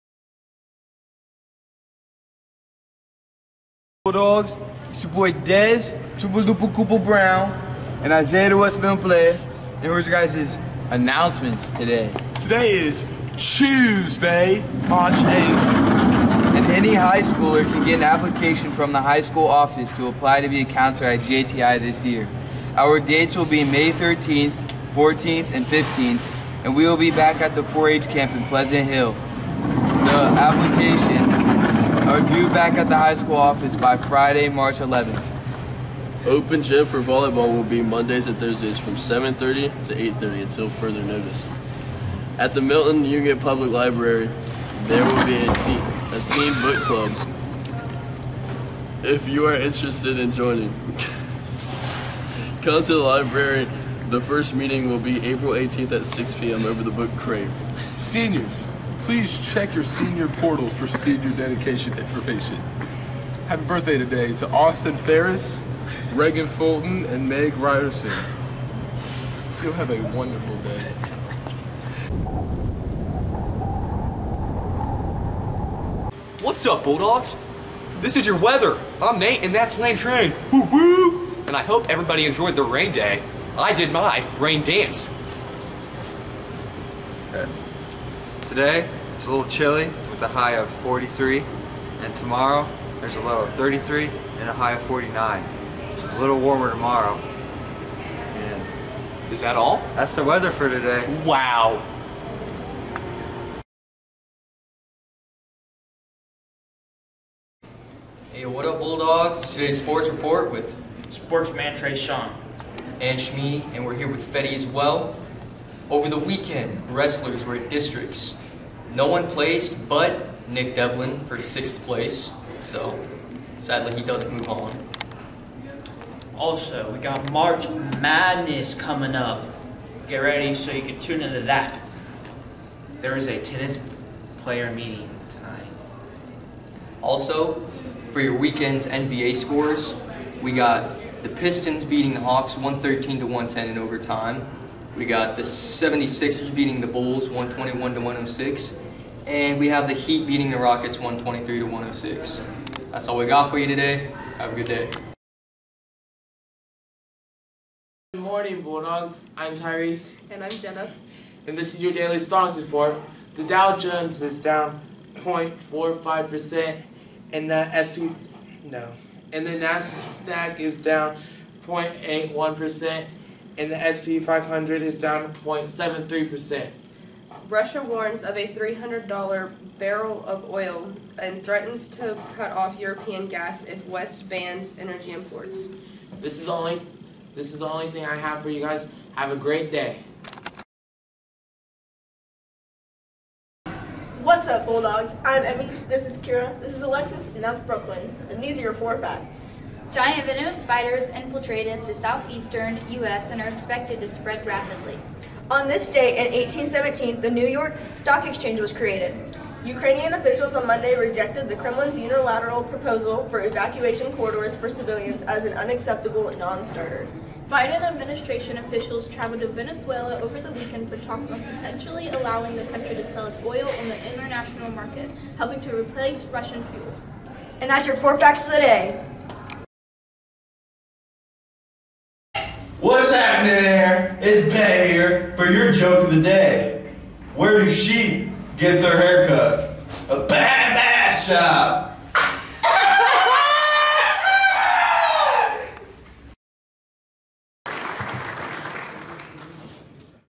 Milton-Union High School Announcements Tuesday February 8th, 2022
MU High School Announcements 3/8/2022:
Announcements, weather, sports, stock report, facts of the day, and joke of the day.